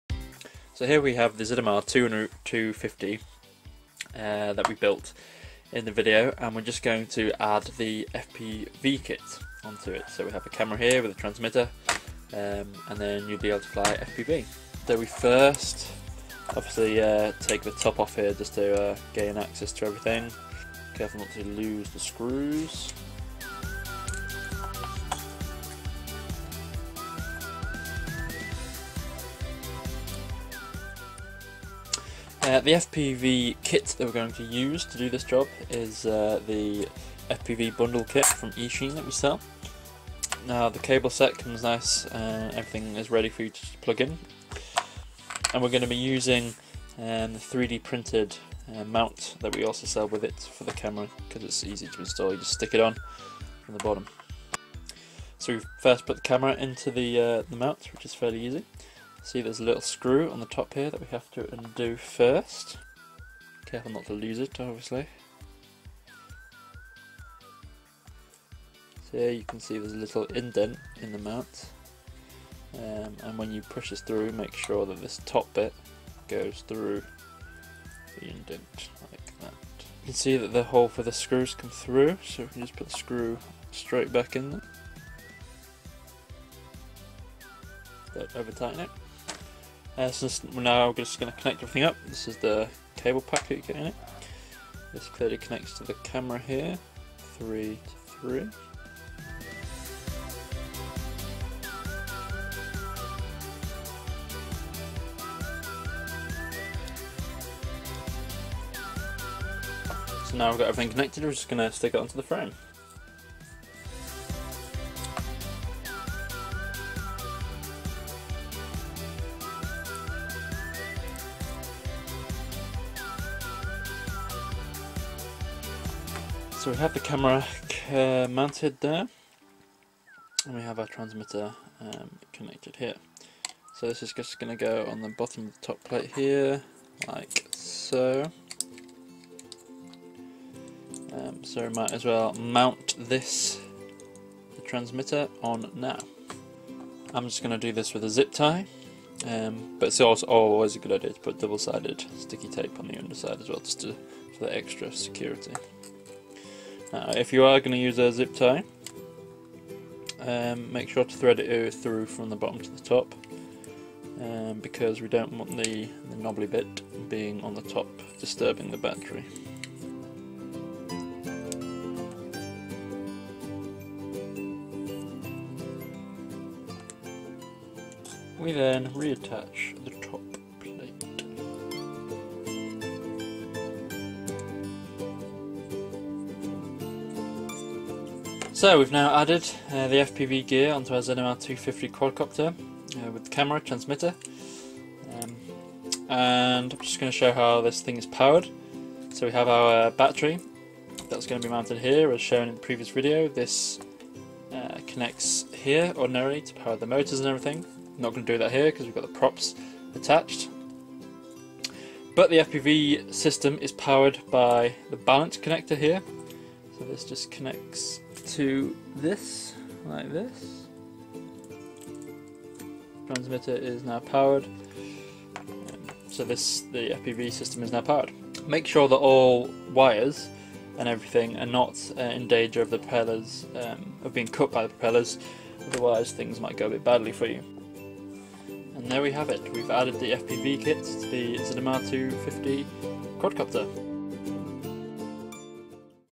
Adding FPV to the ZMR250 Quadcopter
This is a short step-by-step guide for beginners showing you how to add FPV gear onto your ZMR250 quadcopter. Even though we do demonstrate the process with the ZMR250, the steps can be applied easily to all other racing quads.